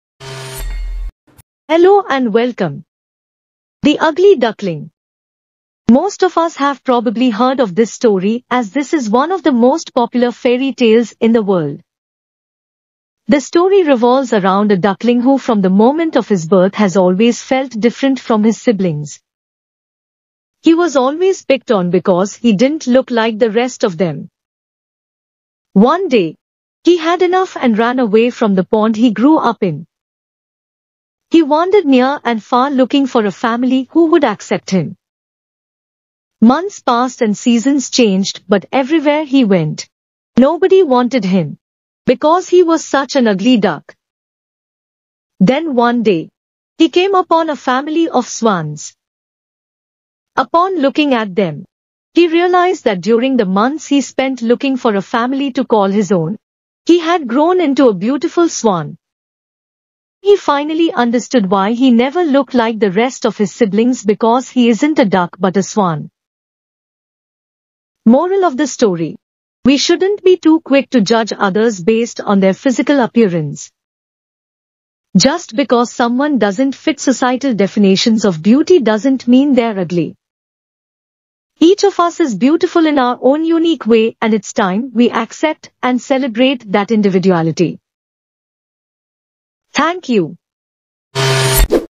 The Ugly Duckling || English Through Stories
Most of us have probably heard of this story as this is one of the most popular fairy tales in the world. The story revolves around a duckling who from the moment of his birth has always felt different from his siblings. He was always picked on because he didn’t look like the rest of them. One day, he had enough and ran away from the pond he grew up in. He wandered near and far looking for a family who would accept him. Months passed and seasons changed but everywhere he went, nobody wanted him because he was such an ugly duck. Then one day, he came upon a family of swans. Upon looking at them, he realized that during the months he spent looking for a family to call his own, he had grown into a beautiful swan. Now he finally understood why he never looked like the rest of his siblings because he isn’t a duck but a swan.

Moral of the story:

We shouldn’t be too quick to judge others based on their physical appearance. Just because someone doesn’t fit societal definitions of beauty doesn’t mean they’re ugly. Each of us is beautiful in our own unique way and it’s time we accept and celebrate that individuality.